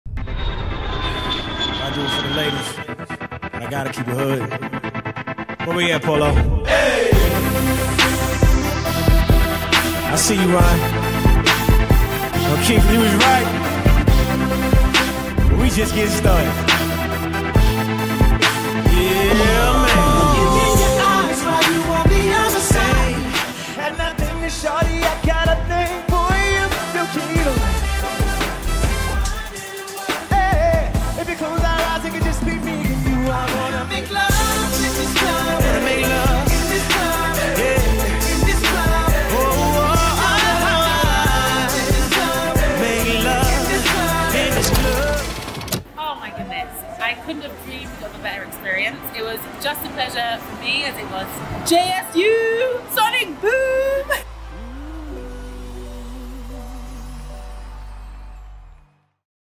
Jackson State University's Sonic Boom Super Bowl Recap
Check out this mini recap from SBOTS Super Bowl Recap where they performed with Usher for the Apple Music's Halftime Show.